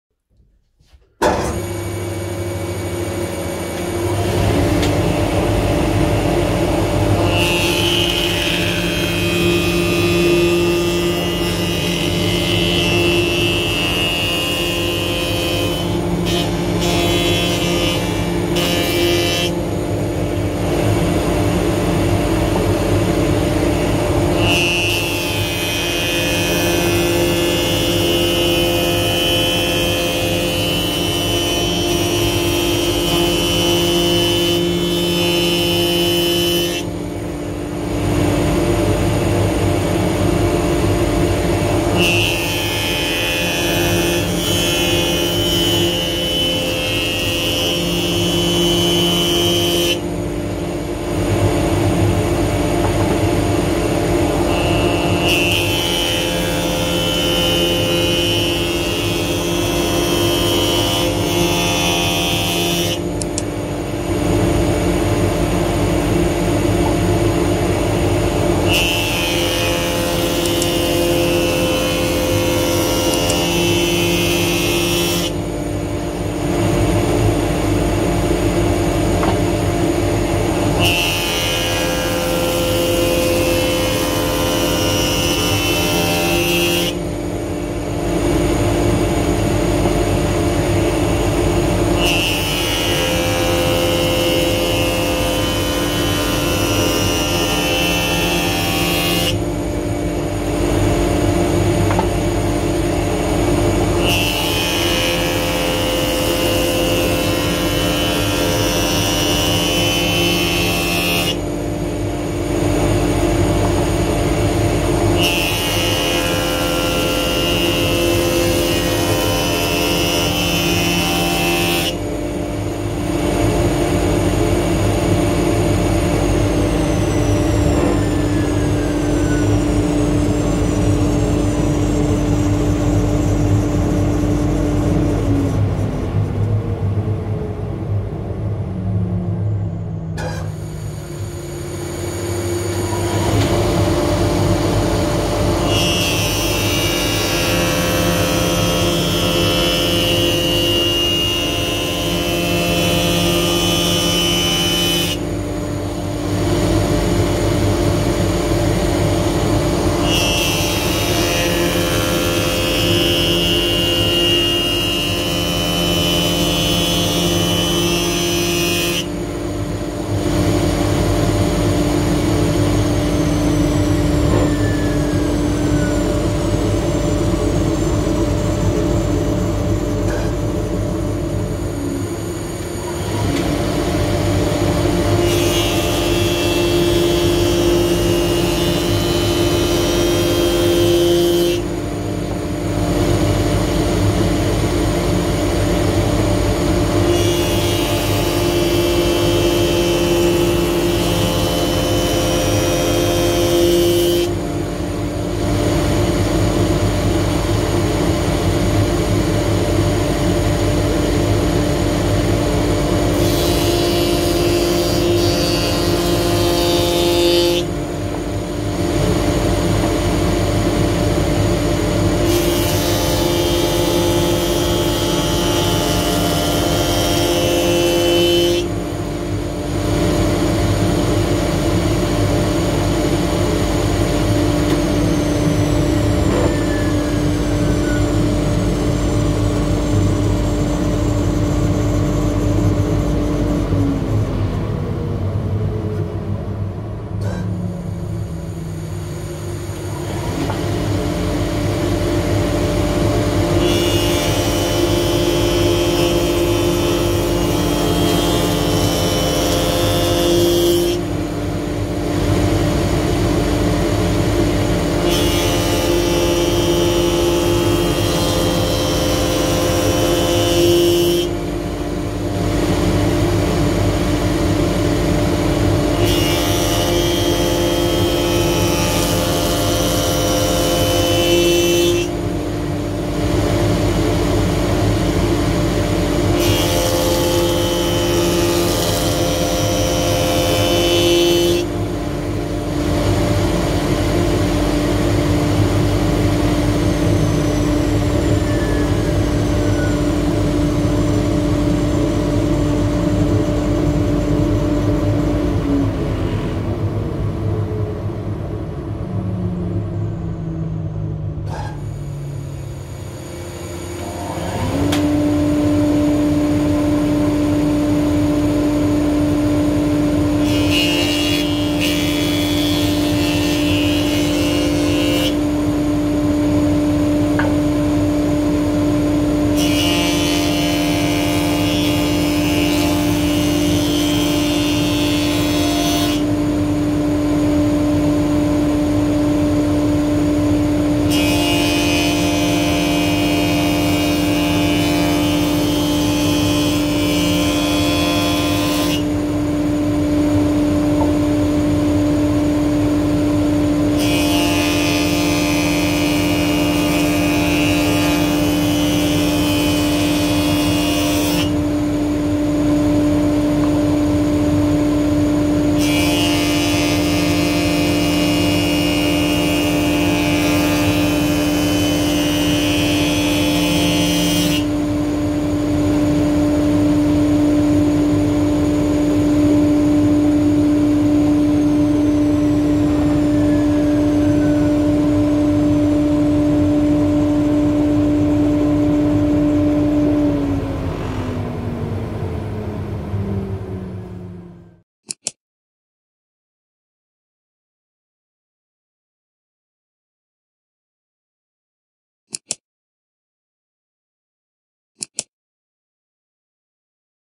Felder Hammer Abricht-Dickenhobelmaschine A3 31
Die Felder Hammer Abricht-Dickenhobelmaschine ist einfach in der Bedienung, mit vielen verstellmöglichkeiten, z.B. eine Winkel verstellung des Anschlages, einstellbar wieviel man vom Holz abhobeln möchte usw.